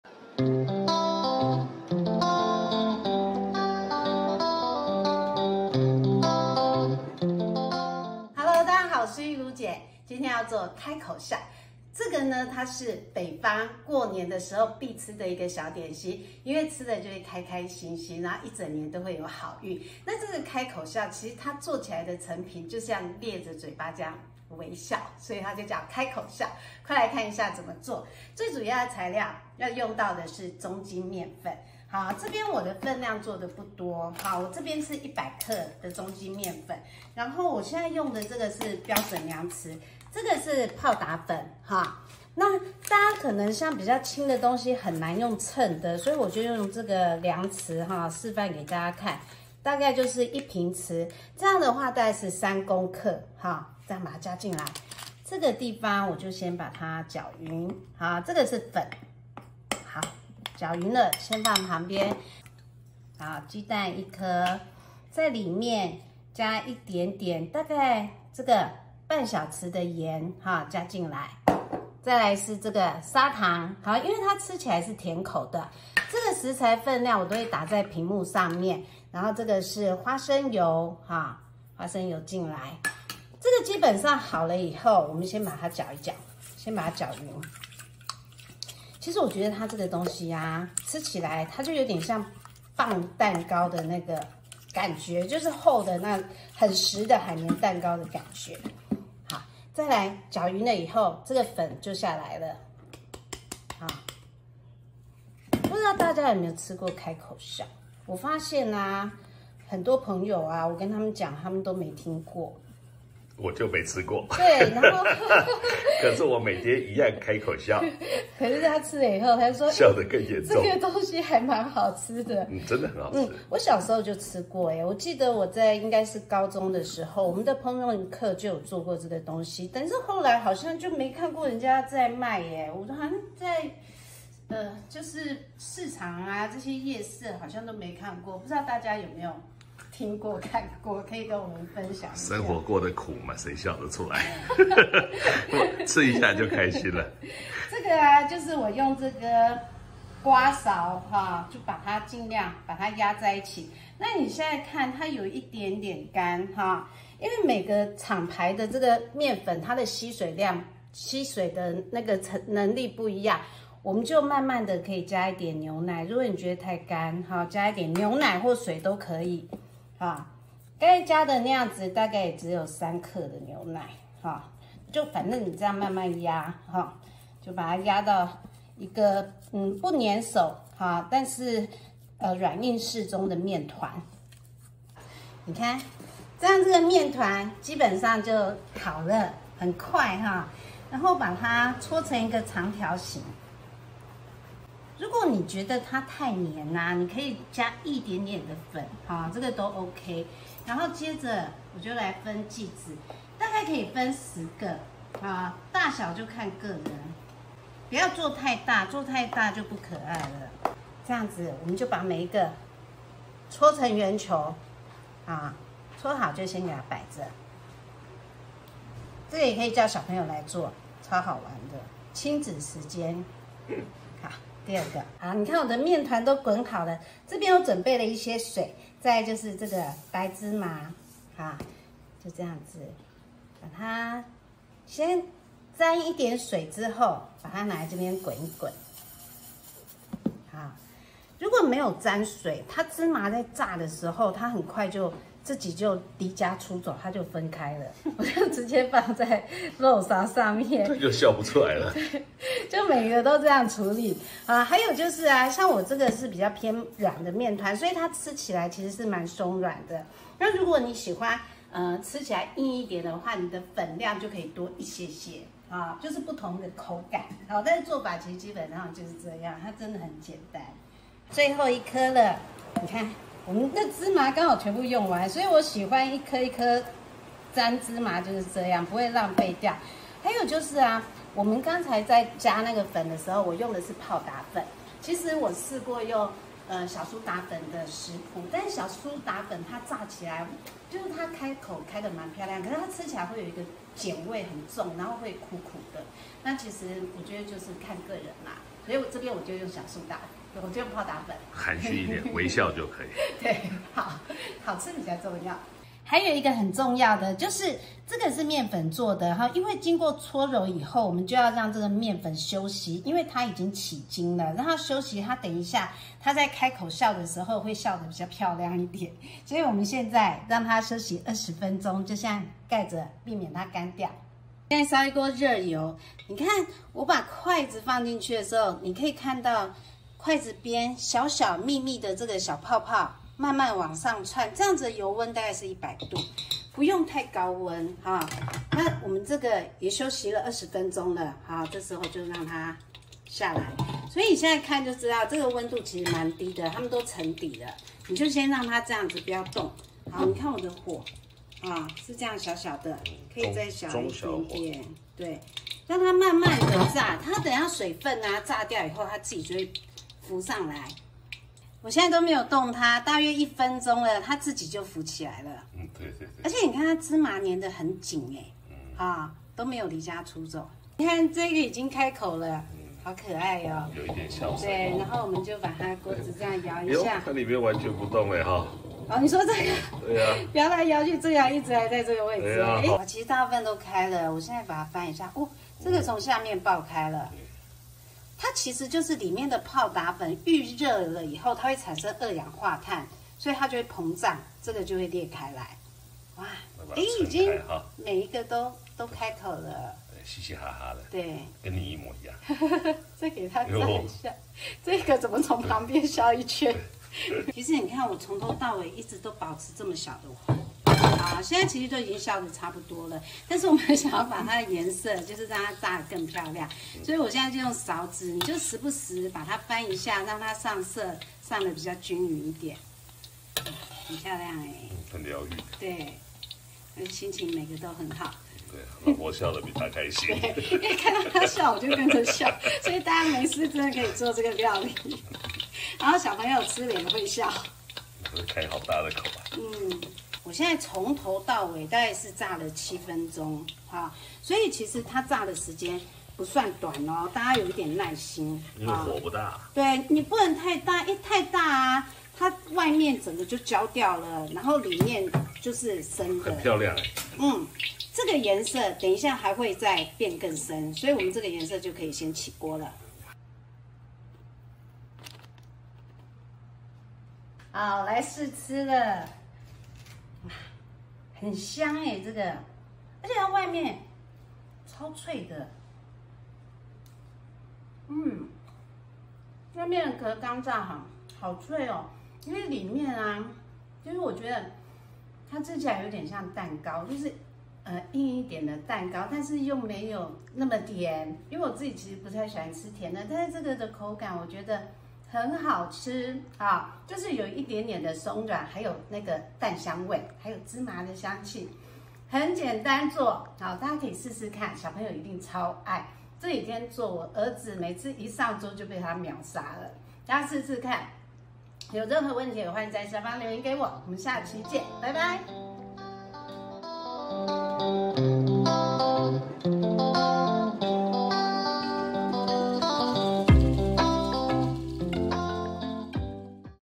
開口笑、保證開口的小秘訣、有人吃過嗎？Smile sesame balls
中筋麵粉 all purpose flour 100g
泡打粉Baking powder 3g
雞蛋Egg 1顆
鹽 Salt 2g
糖 Sugar 25g
油 Oil 10g
牛奶 Milk 3g
白芝麻 White sesame 20g

分成10個小圓球Divide into 10 small balls

重點Tips:‼️
油炸前靜置20分鐘，讓麵糰充分休息，炸的時候才會開口
Let stand for 20 minutes before deep-frying, let the dough fully rest, it will open when deep-frying

麵團如果太濕可以再加一點麵粉
If the dough is too wet, add a little more flour

低溫油炸、大約炸7分鐘
Fry on low heat for about 7 minutes

小蘇打粉的味道會帶一些苦味，如果要用不要加太多
The taste of baking soda powder will bring some bitterness, if you want to use it, don't add too much